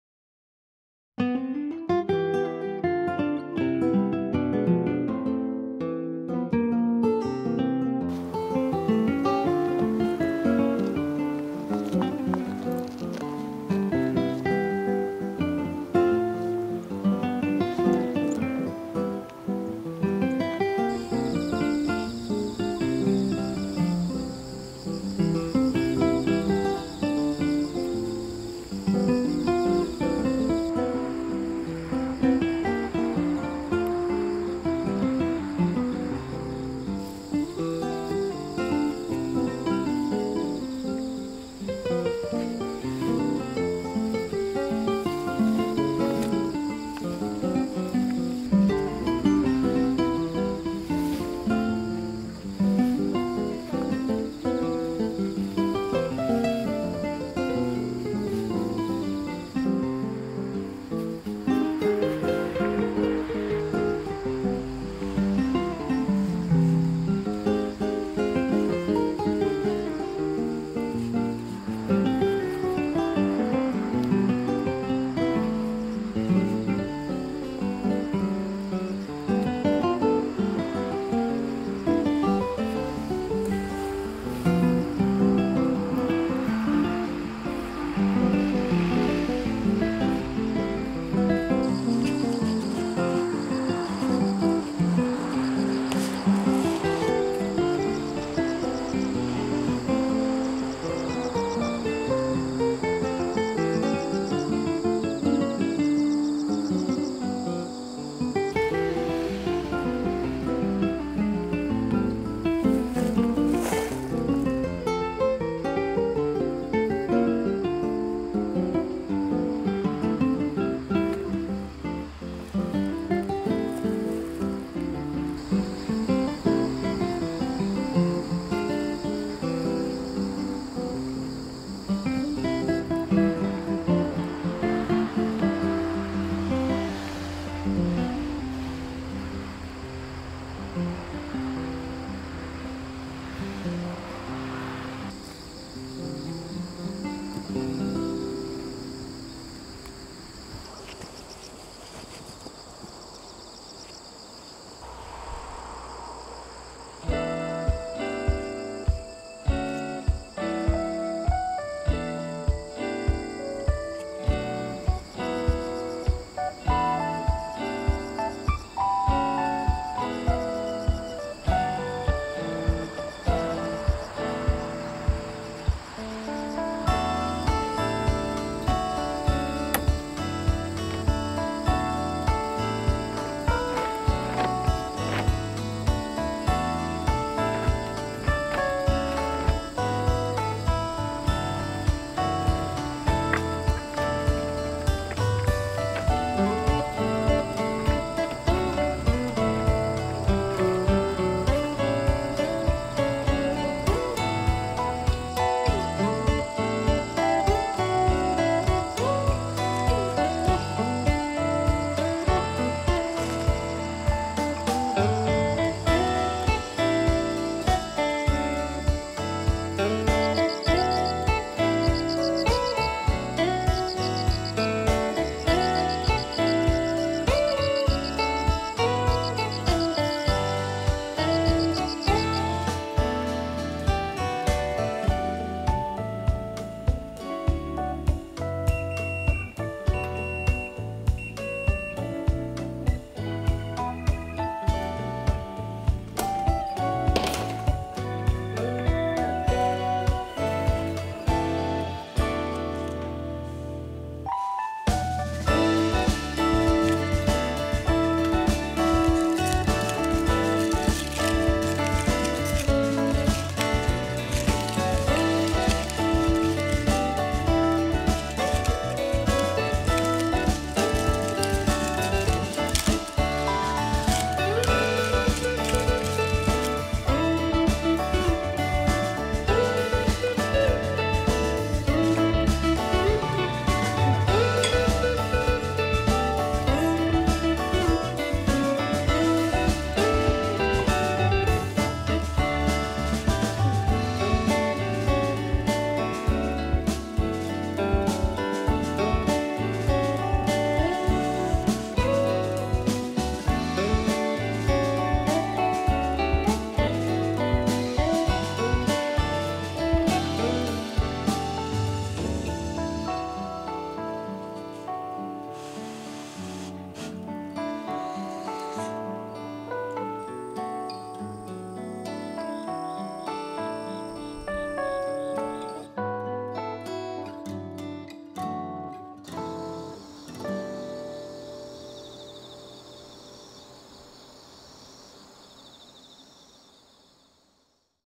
さつまいものグリーンカーテン失敗｜収穫｜実食🍠
さつまいものグリーンカーテン、残念ながら失敗に終わりました。。。
でも美味しいお芋ができたので結果オーライです🥰

🌾🌱🌼動画で登場した園芸用品などをROOMで紹介しております👩‍🌾🌱🌼

日々の日記的な内容です🏃‍♀️園芸とかご飯とかキャンドルとか